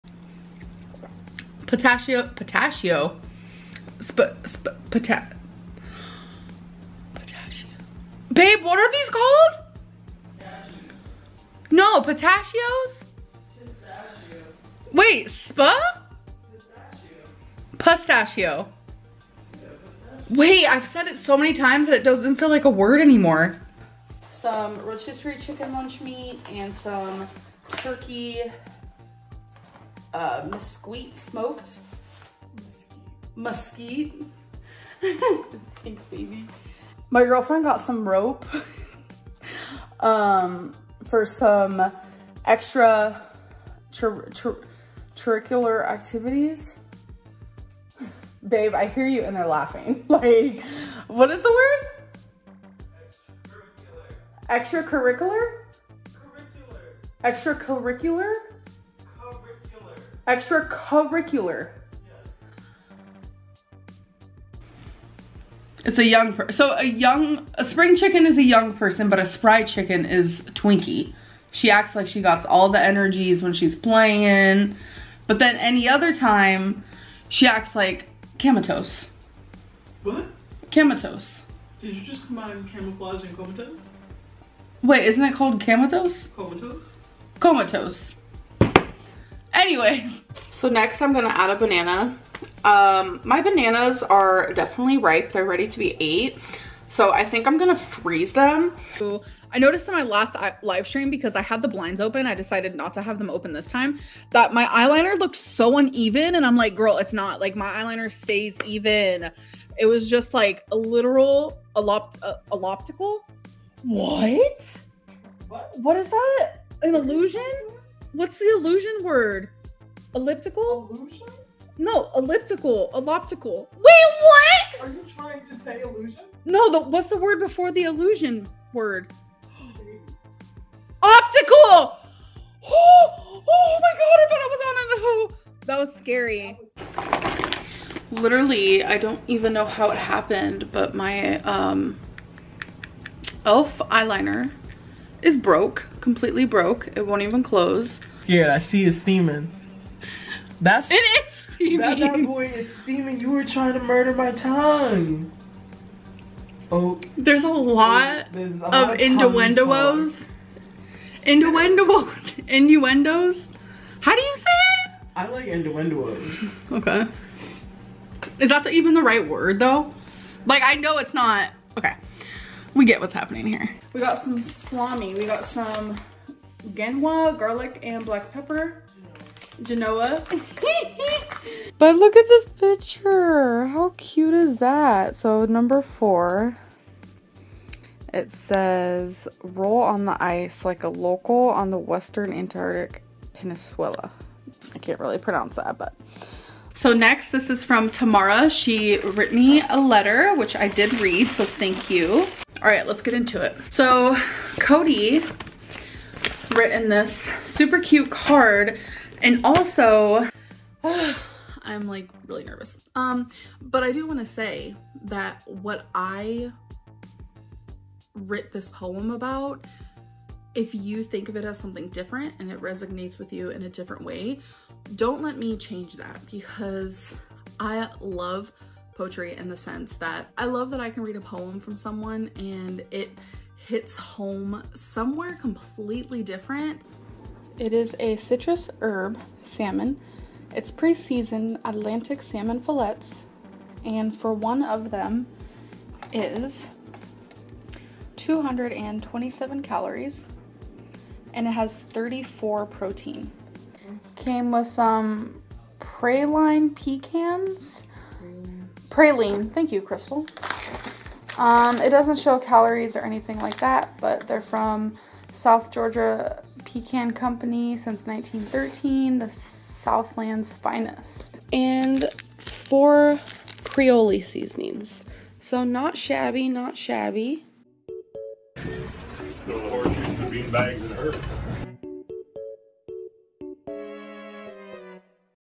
Amberlynn Reid can't pronounce these big words
or pretending? Amberlynn is a self-proclaimed great writer and poet with 140 iq, but sometimes can't pronounce words and has bad grammar